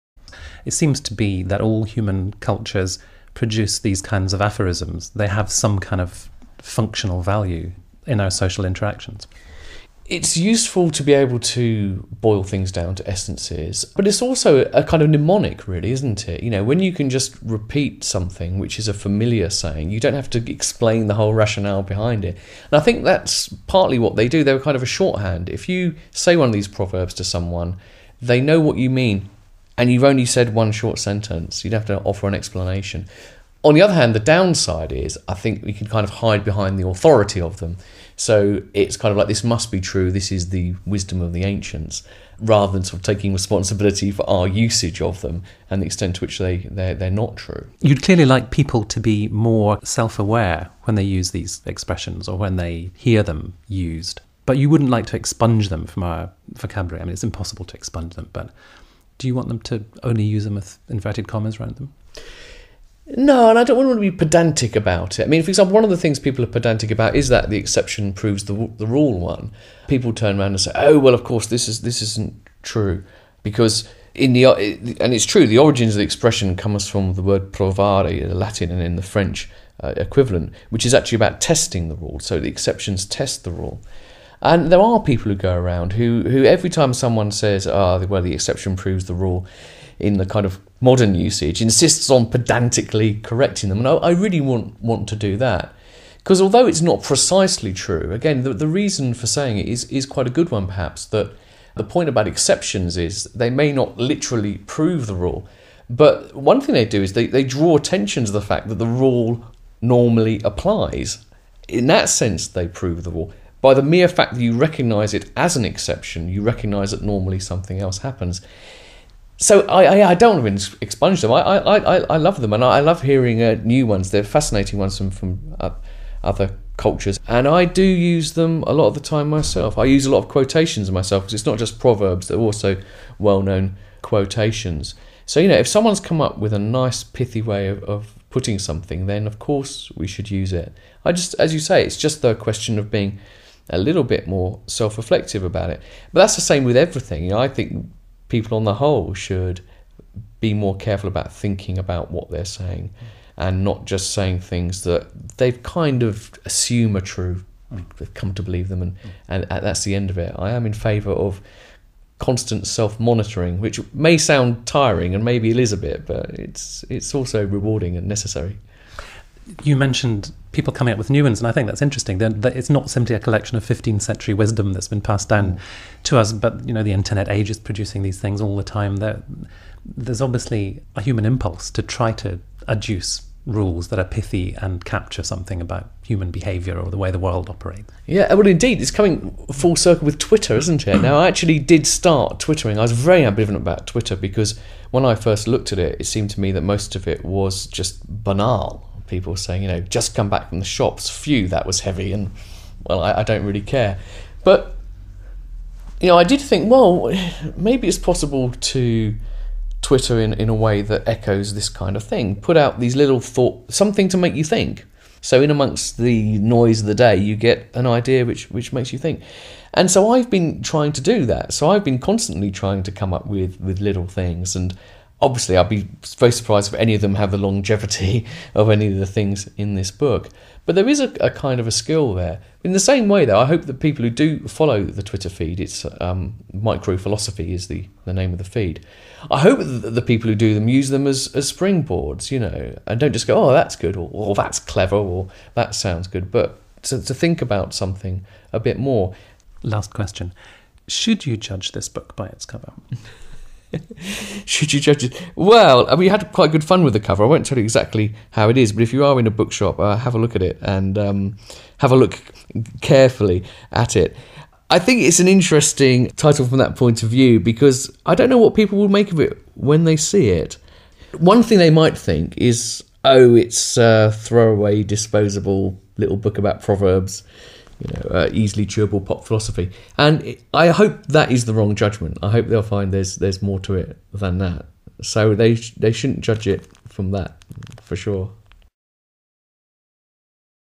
Julian Baggini - Should You Judge This Book by Its Cover? - Part 2 of 2
Julian Baggini offers philosophical opinions to 100 familiar saying and quotations such as 'is the grass always greener on the other side?' 'Is there ever smoke without fire?' And 'is it better to be safe than sorry?'